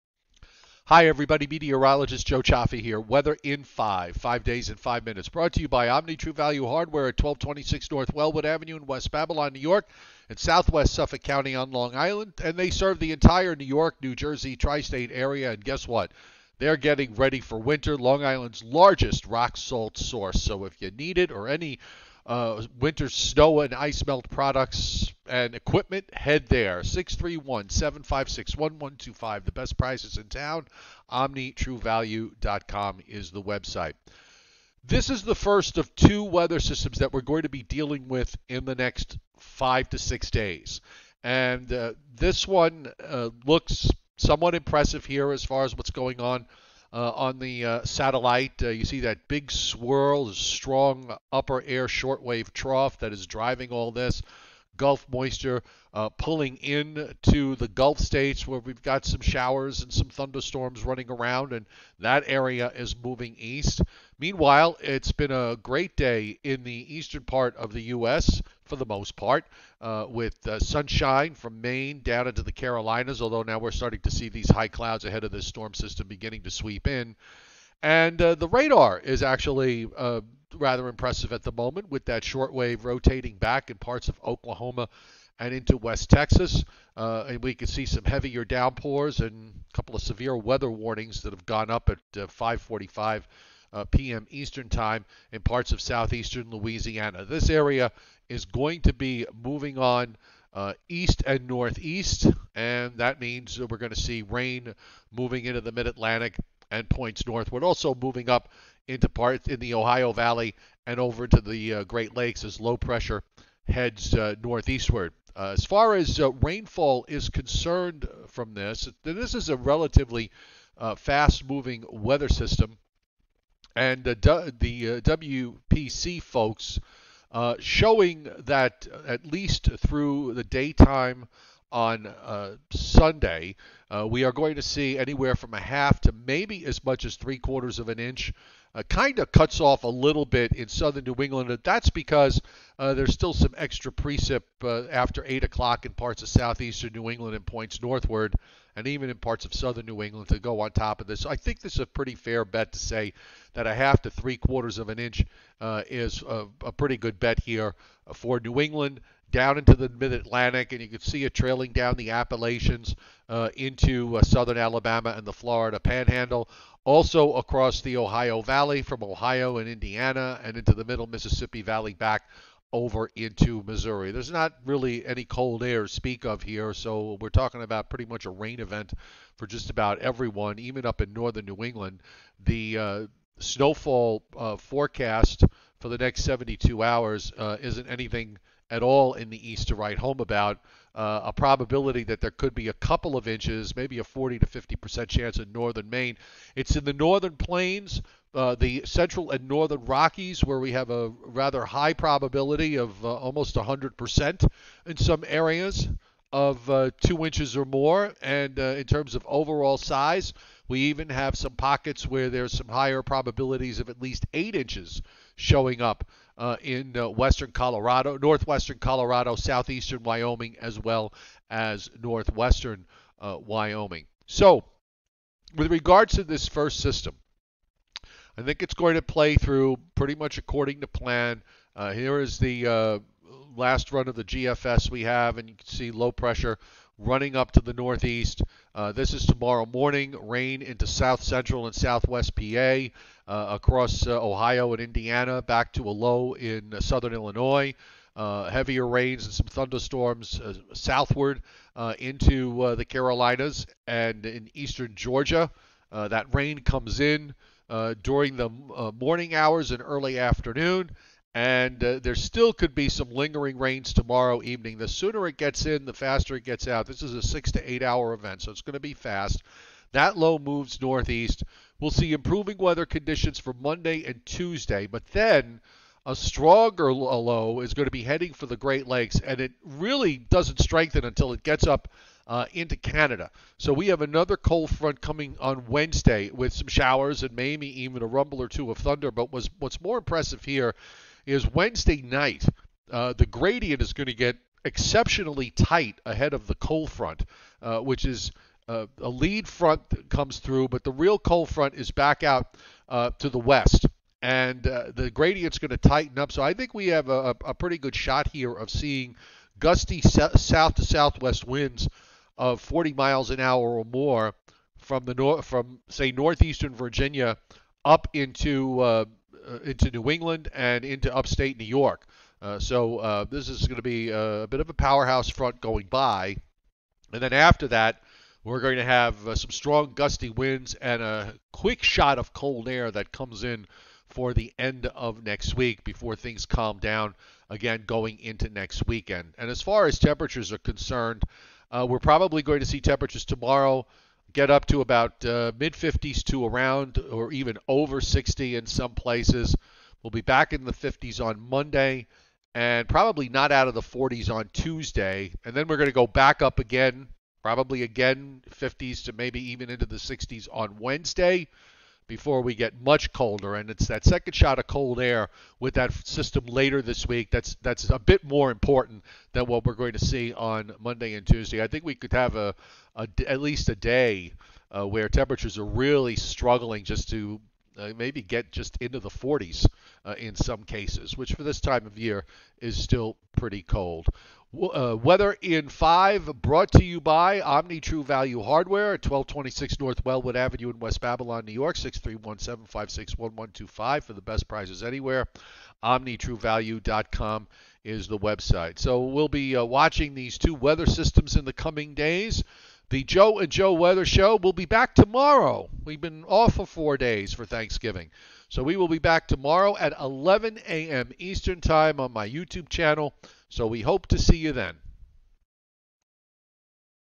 Weather in 5 Two Storm Systems and Cold Fronts In the Next 7 Days Wind Issues Wednesday Northeast
Weather in 5 Sunday brings the first of two weather systems that will produce rain from the Ohio Valley and up and down the East Coast. A thunderstorm or two could be in the mix. A shot of colder air starts the week in the Northeast and Mid Atlantic before another stronger storm heads for the Great Lakes and Southeast Canada Wednesday into Thursday. This system could produce showers, thunderstorms and rather strong winds from the Northern Mid Atlantic to New England.followed by another quick shot of cold air for the end of the week.

Omni True Value Hardware has everything you need to make sure your home is safe this coming winter